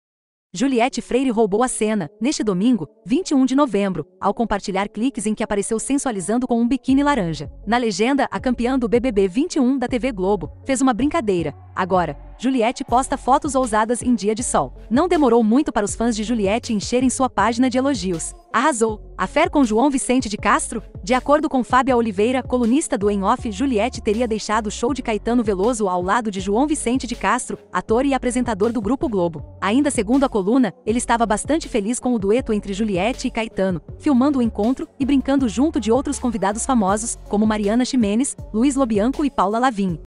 Juliette Freire Sensualiza de Biquíni Laranja
Juliette Freire roubou a cena, neste domingo